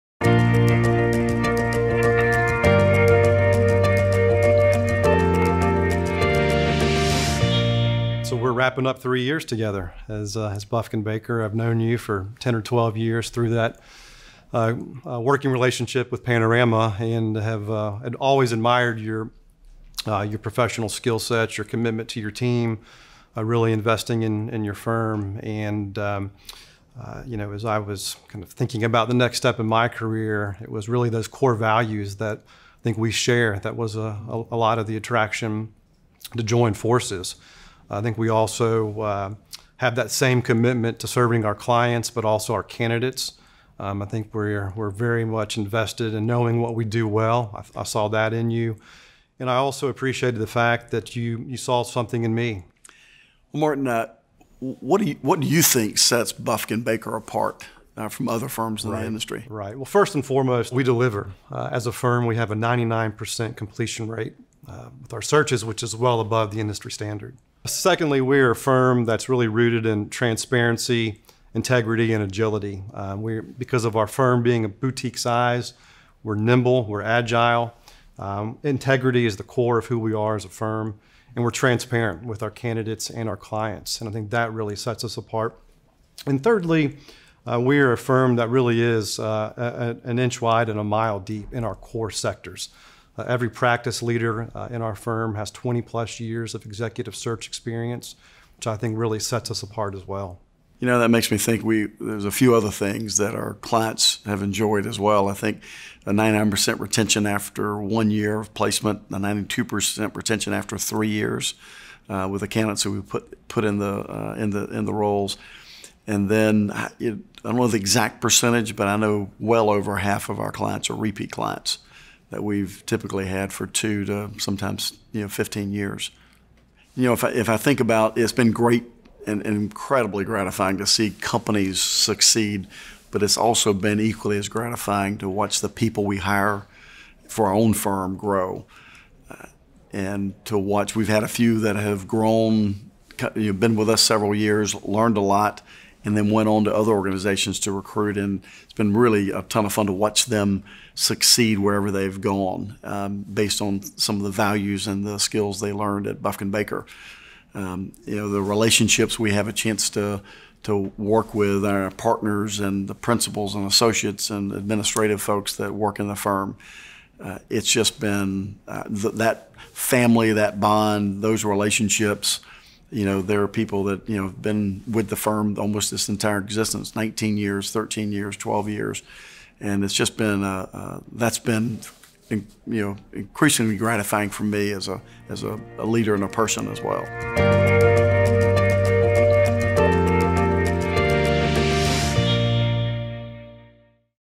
Bufkin / Baker and the Traits of Our Firm
What sets Buffkin / Baker apart from the others?

Buffkin / Baker is a partner-led executive search firm focusing on healthcare, higher education, private equity, entertainment/media, digital, and non-profit.  As leaders in their respective fields, each partner has over a decade of retained search experience. Combined, the firm's partners have more than 175 years of search experience and more than 220 years of industry/operating experience. Buffkin / Baker is a member of Panorama, a global community of over 400 leadership experts working together to promote a diverse perspective of leadership, embracing creative thinking, and sharing our expertise and experience to bring our clients the very best results.